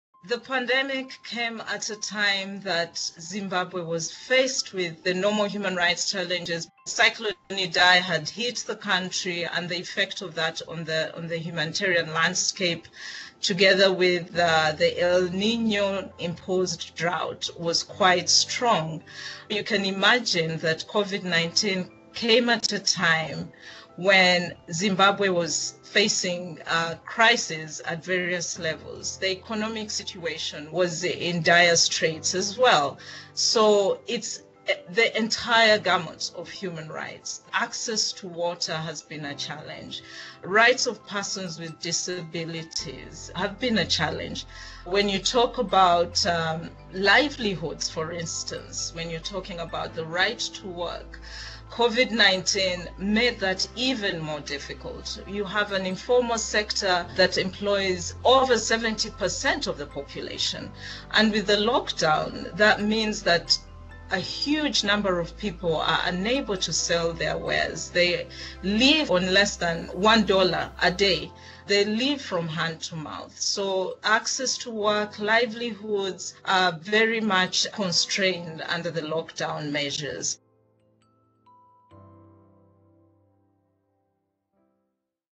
Voices from the Field - Zimbabwe
“While the pandemic is primarily a public health crisis, it has impacted the entire gamut of human rights and if we do not stand together to fight to protect all human rights for everyone now, we will not be able to successfully win the war against the pandemic,” says Fiona Adolu, Human Rights Adviser in Zimbabwe.